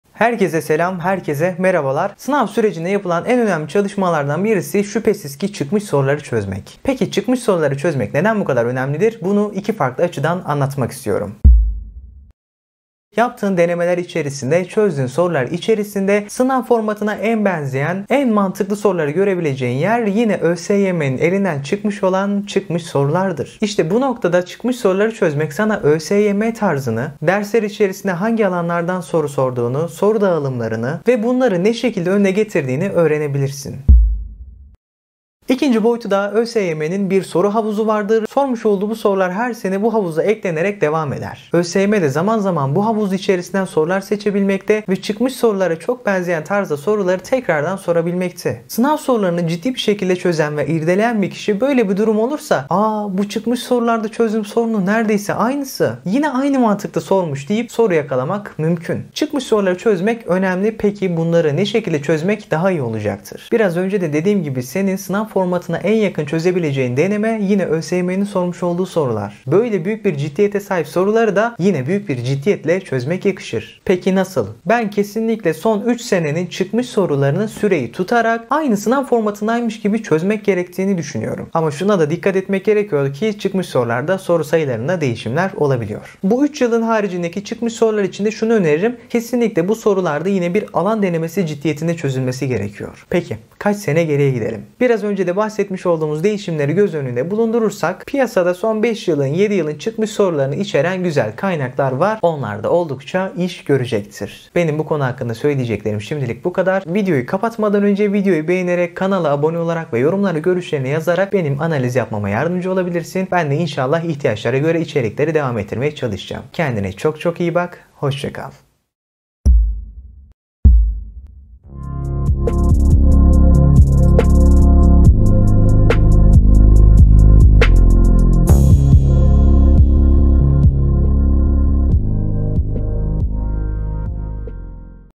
KPSS ÇIKMIŞ SORULAR NASIL ÇÖZÜLMELİ - KAÇ SENE GERİYE GİDİLMELİ?
Bu videomda Kpss çıkmış soruları hakkında konuştum. Çıkmış sorular nasıl çözülmeli, çıkmış soruları çözmenin faydaları ve kaç sene geriye gidilmesi gerektiği hakkında bilgiler verdim. İyi Seyirler.
Diğer videolardan anında haberdar olmak için bildirimleri mutlaka açınız.
 Bu videoyu arkadaşlarınıza göndererek onlara da destek olabilirsiniz :)